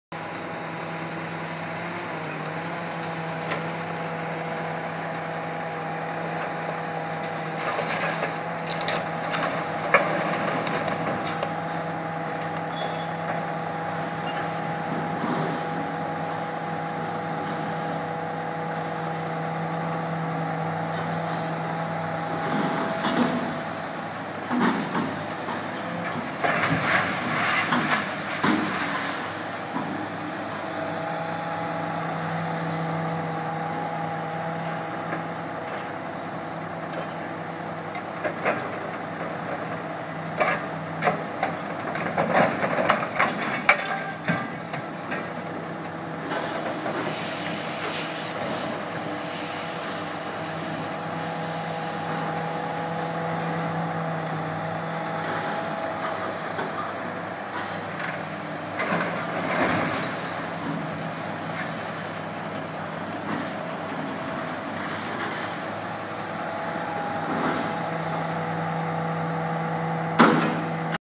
Excavator unloads 2 more cooling towers
2 more towers to the scrap pile